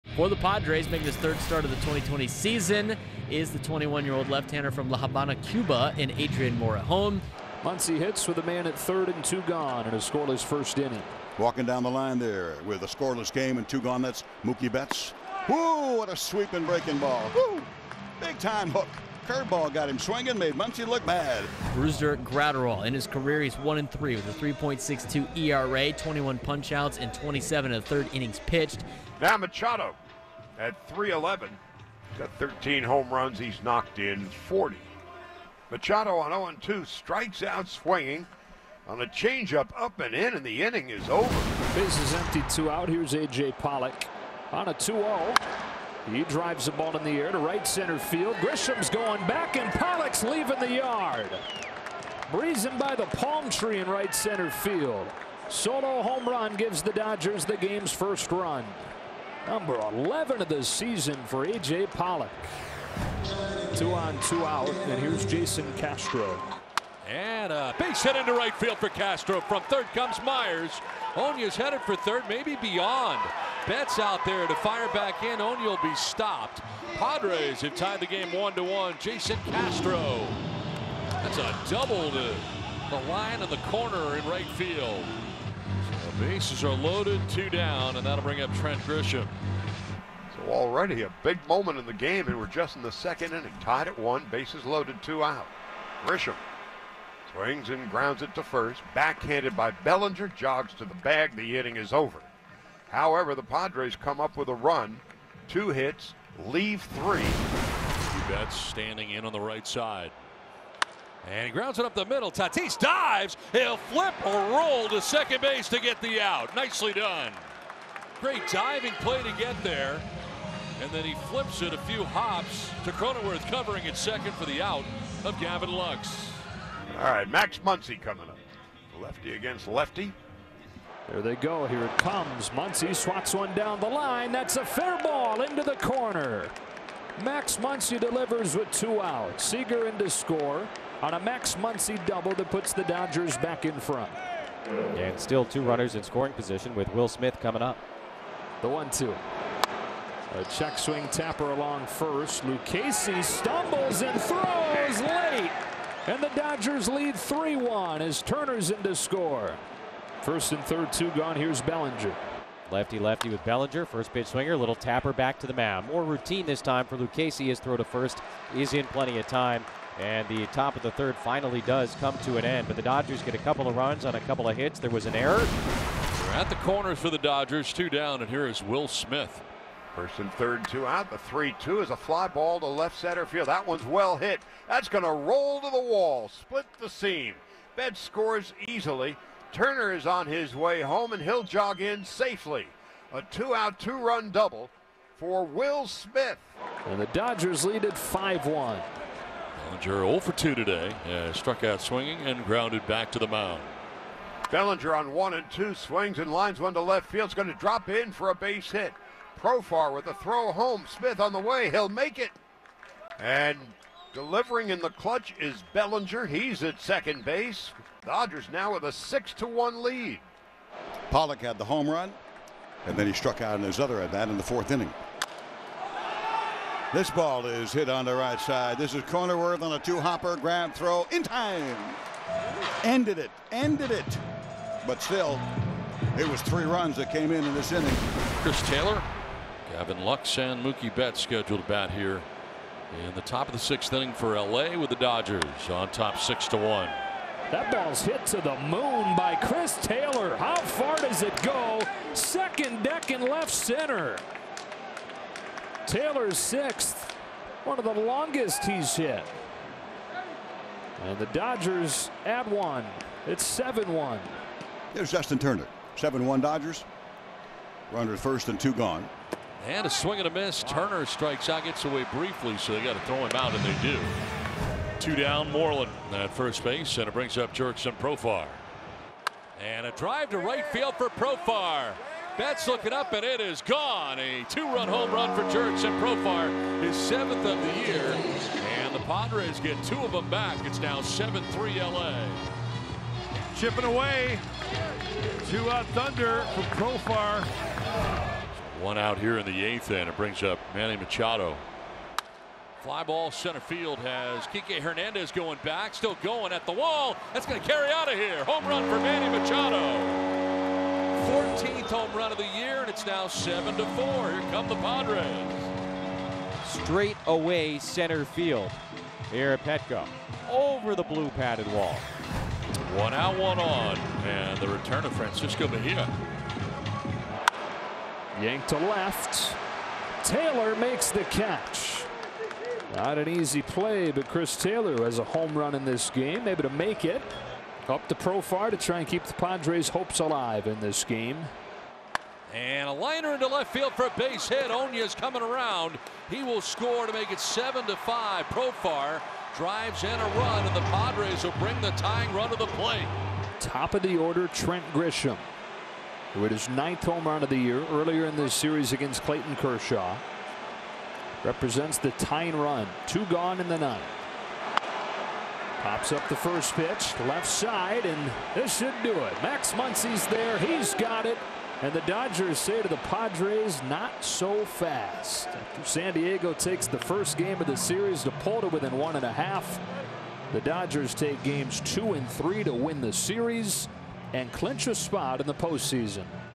Dodgers clinch postseason berth in a 7-5 win | Dodgers-Padres Game Highlights 9/16/20
The Dodgers' offense powered a 7-5 win against the Padres to become the first team to clinch a postseason berth this year